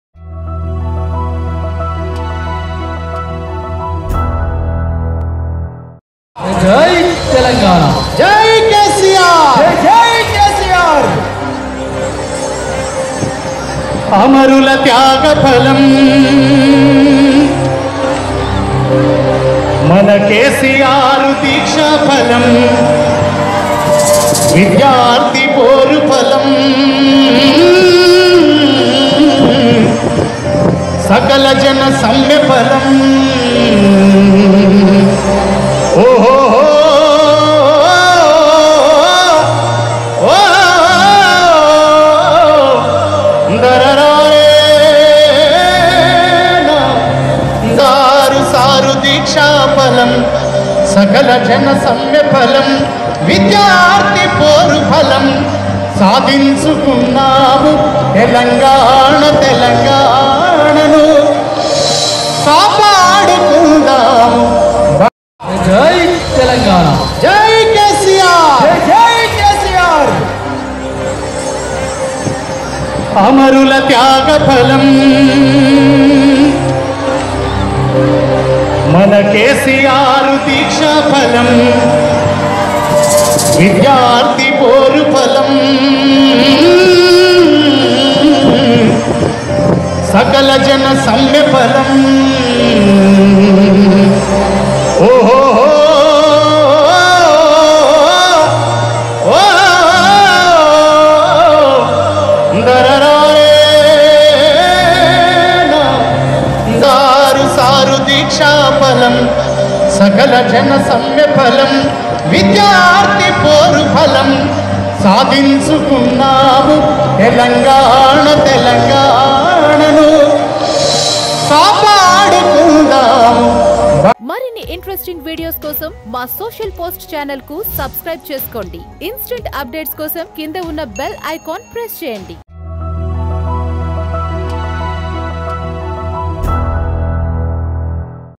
KCR Live | CM KCR's Praja Ashirvada Sabha Live | TRS first Election Campaign LIVE
Praja Ashirvada Sabha In Husnabad LIVE | CM KCR Speech | Pre Polls In Telangana |